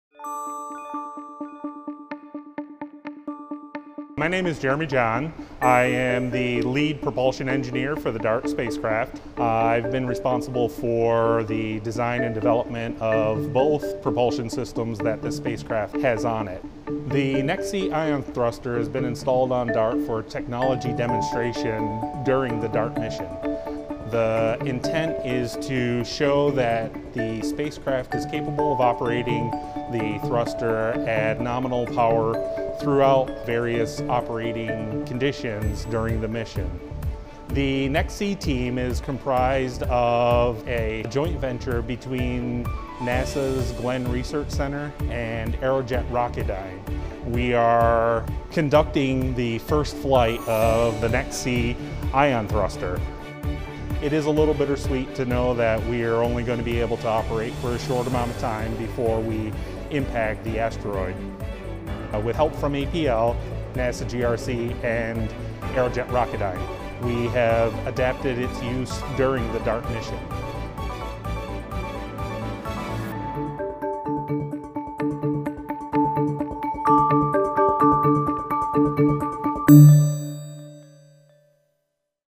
See the NASA DART spacecraft's ion thruster get installed
NASA's Double Asteroid Redirection Test (DART) sports the NEXT-C (NASA's Evolutionary Xenon Thruster-Commercial) ion propulsion system. Watch it get installed.

Credit: JHUAPL